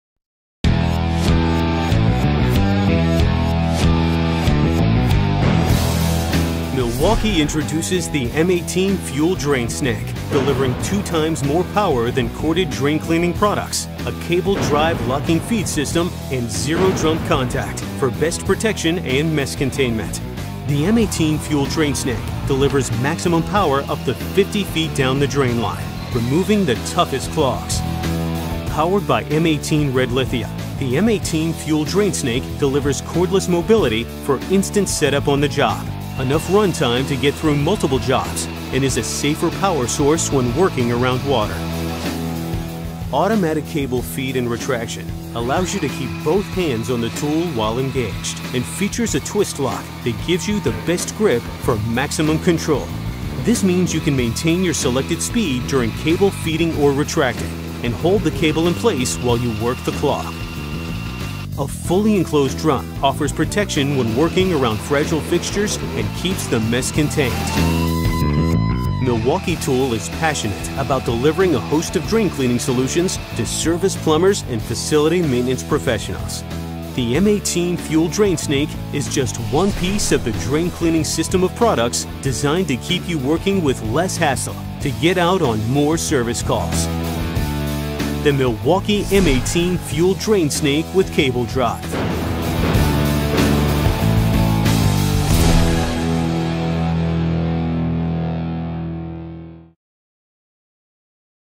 STURATUBI MILWAUKEE A BATTERIA M18 - PRODOTTIFERRAMENTA
Lo STURATUBI più compatto ed ergonomico del mercato

Il design ibrido lo rende adatto ad essere utilizzato anche in appoggio su un piano

Maggiore flessibilità e manovrabilità rispetto ad utensili a filo

Fornito con spirale flessibile del diametro di 8 mm, per l'utilizzo con i tubi di scarico fino a 50 mm (compatibile sia con spirale ø 6 sia con ø 8)